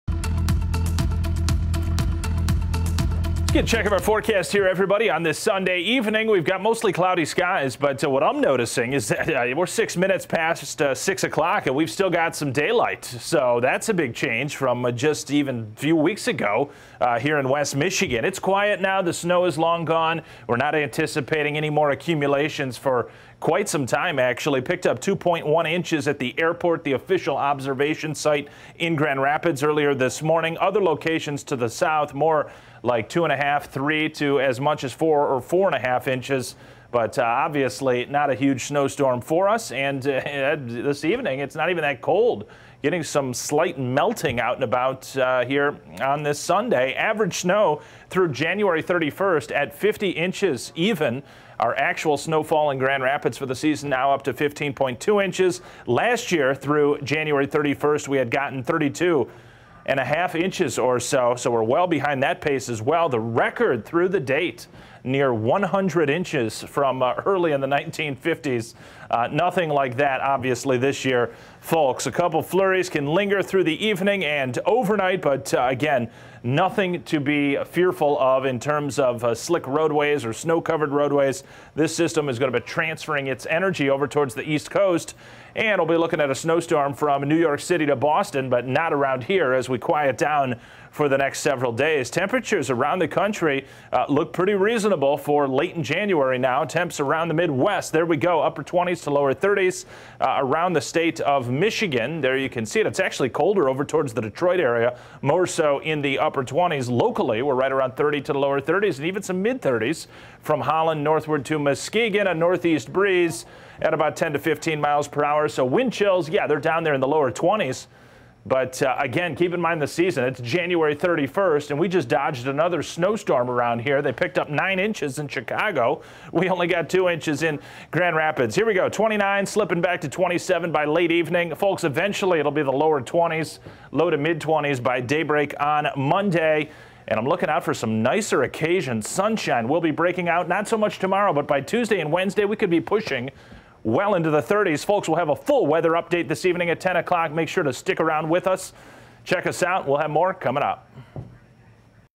Sunday Evening Forecast Jan 31, 2021
Quieter weather week ahead